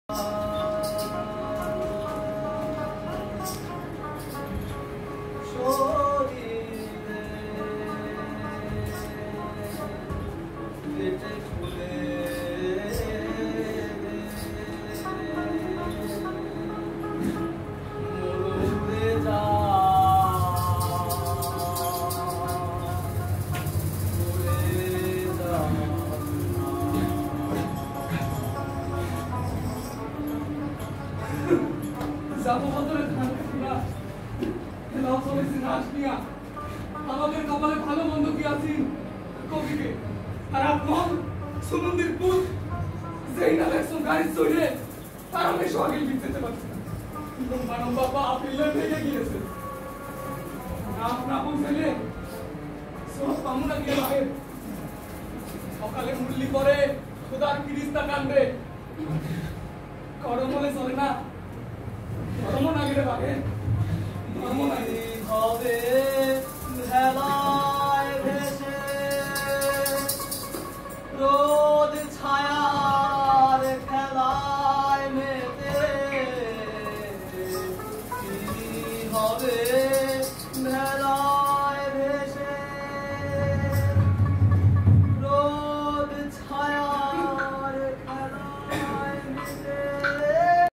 Chaka Natok। চাকা নাটক
This stage performance   is under of  course production students of Jatiya kabi kazi Nazrul Islam university trishal  Department of TPS  16-17 session...  So enjoy the video.